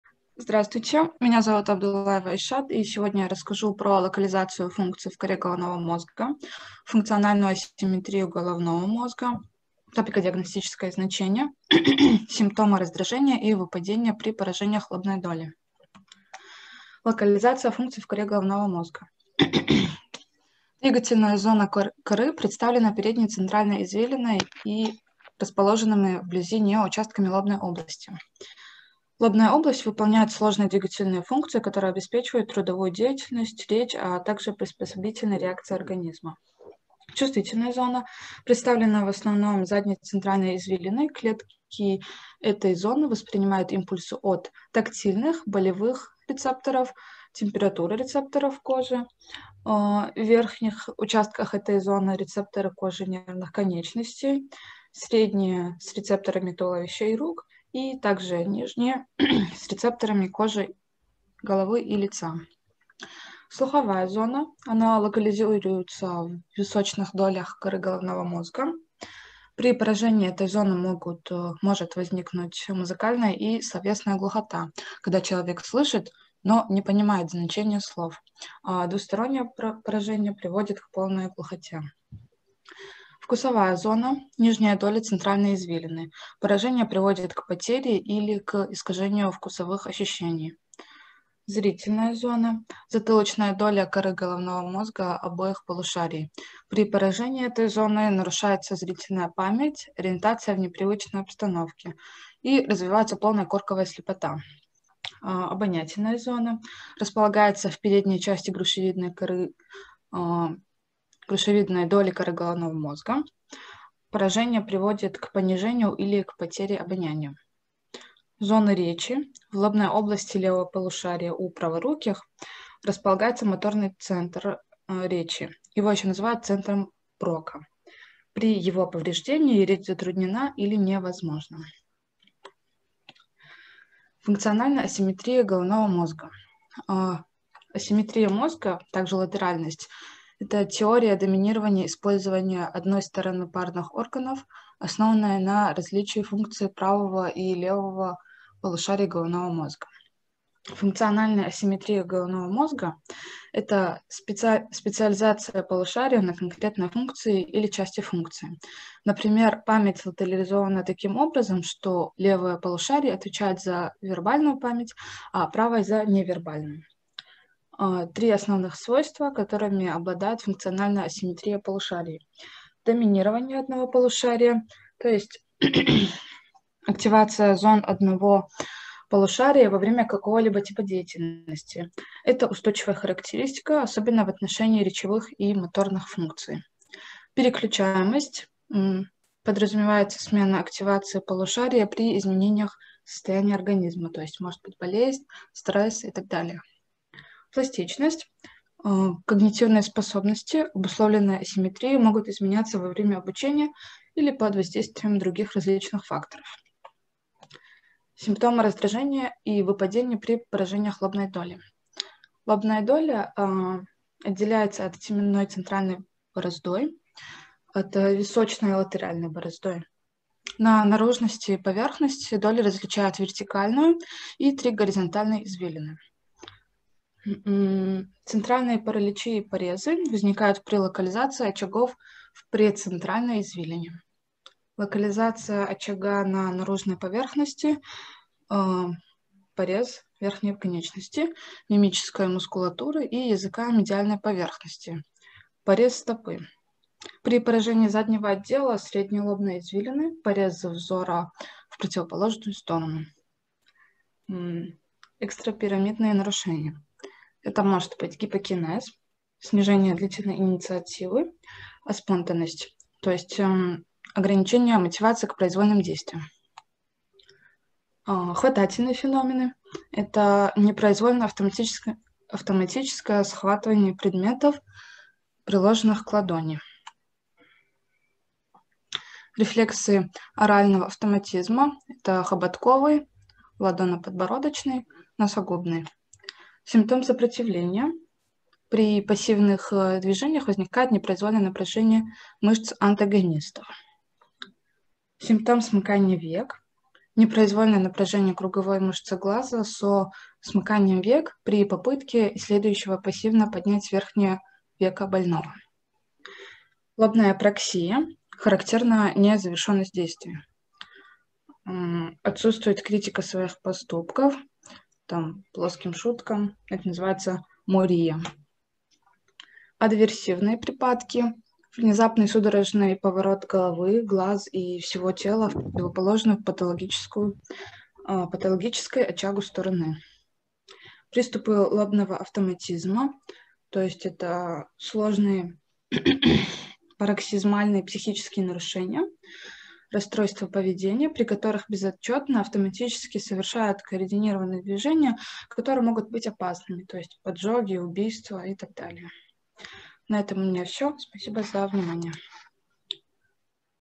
23 Кора Лобная доля
Студенческая работа. Экзаменационные ответы по неврологии / Student work. Exam answers in neurology / عمل طالب. إجابات الامتحان في علم الأعصاب / Travail étudiant. Réponses aux examens en neurologie